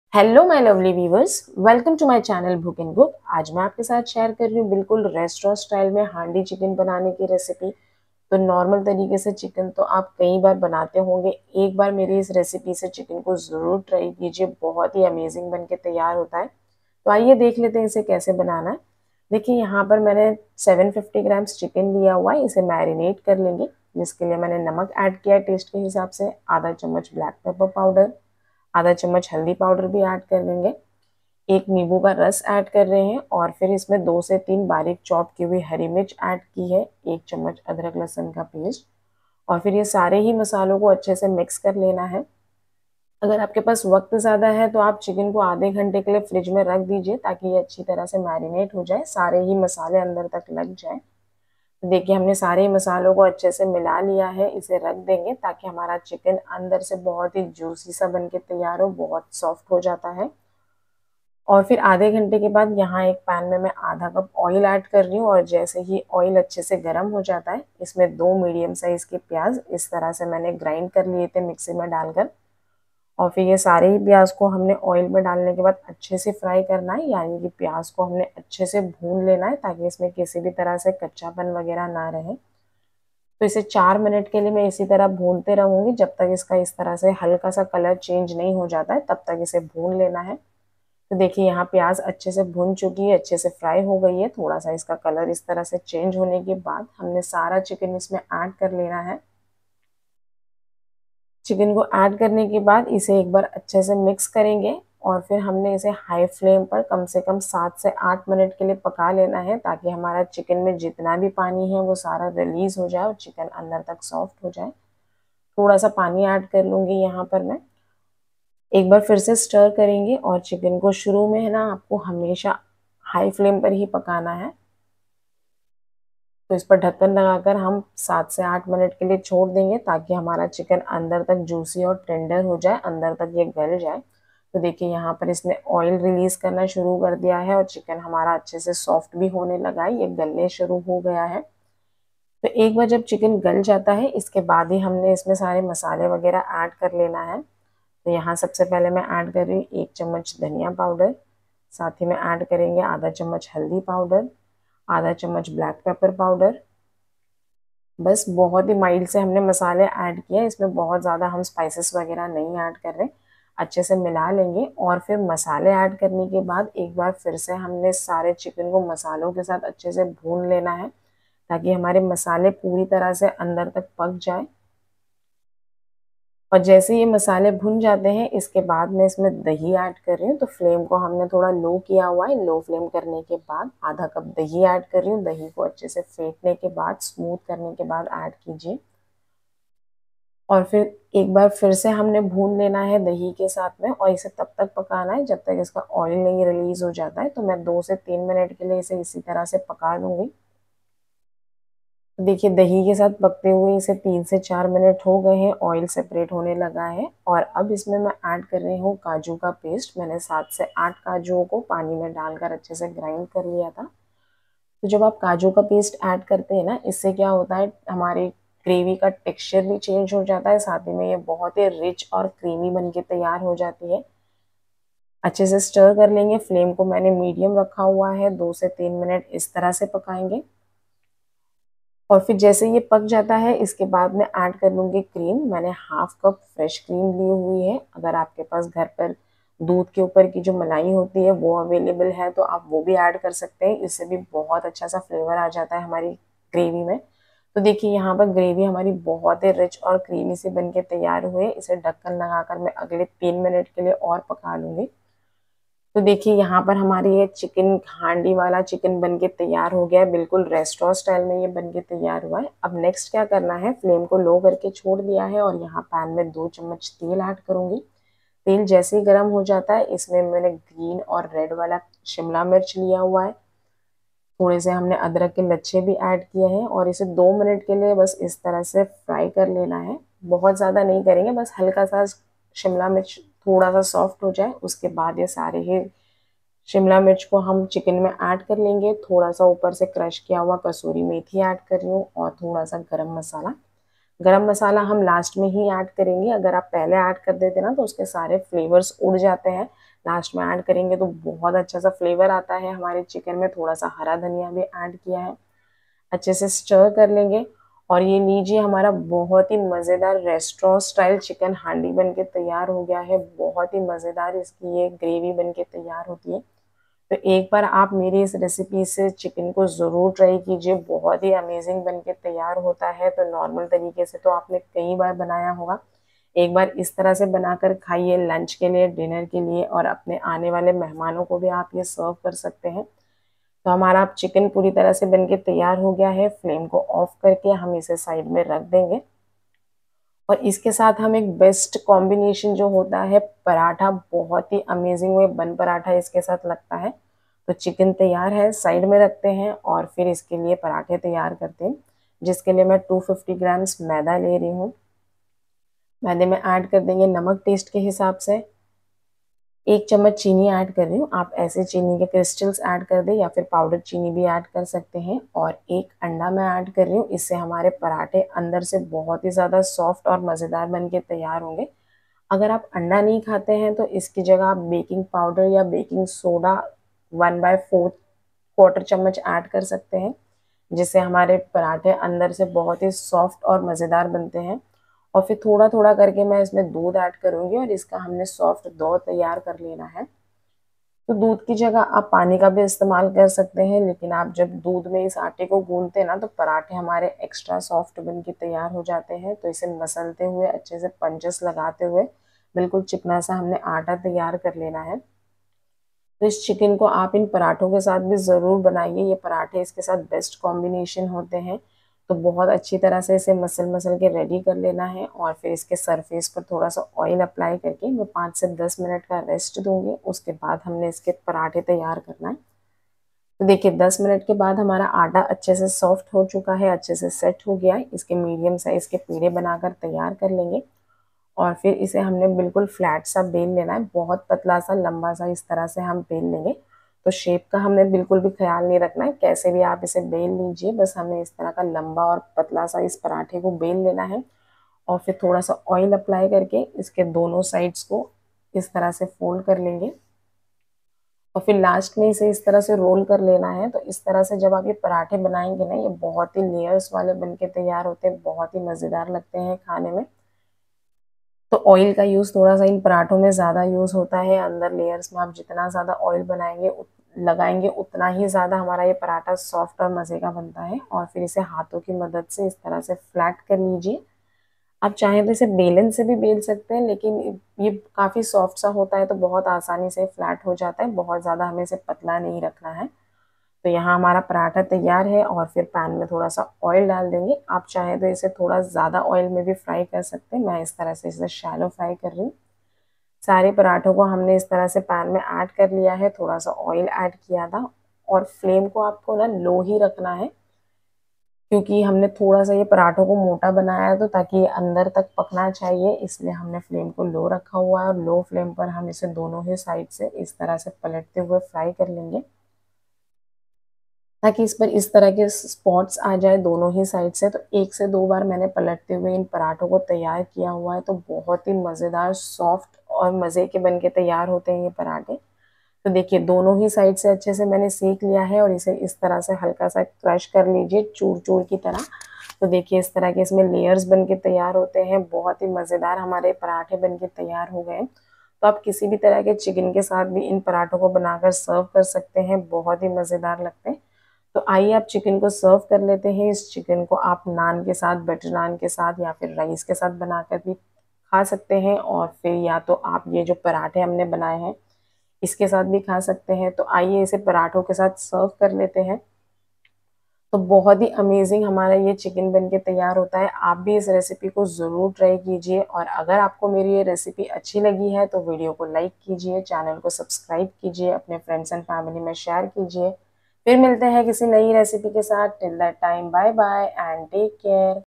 Chicken Handi Recipe | Chicken Recipe With Paratha | How To Make Restaurant Style Chicken At Home
Hi Friends,
This is Bhook N Cook. Perfect destination for your hunger and taste.

In this video I am going to show you how to make restaurant style chicken handi at home. It is an easy chicken recipe with less ingredients. Chicken ka salan banane ka aasan tarika bataya hai es video me. Chicken with bun parotta can be made for lunch recipes. It is an easy chicken gravy recipe which can be made as main course for any special ocassion. Do try this recipe, I hope you will love this chicken recipe.